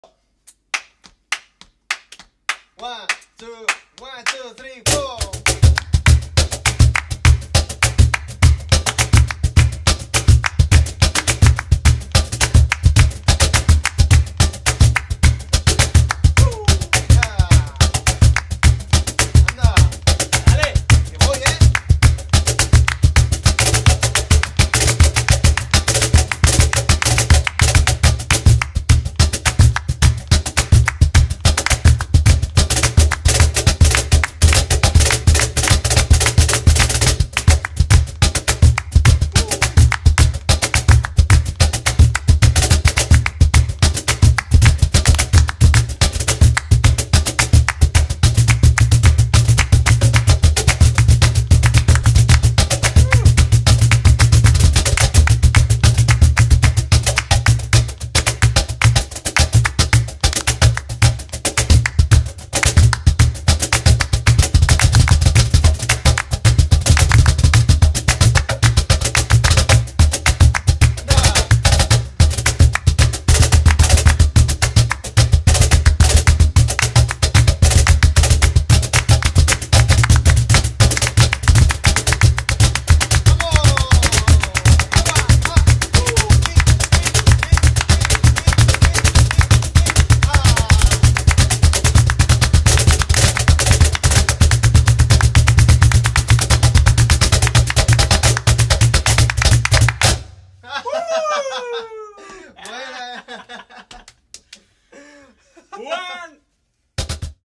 CAJON JAM - Felipe Fagundes + Juan Toro (Rumba Flamenca)
Full CAJON JAM with my friend Juan Toro, Lots of Fun!!!
To learn the main groove watch the cajon tutorial Rumba Flamenca

☞ All mics I use:
Shure SM57
AKG C3000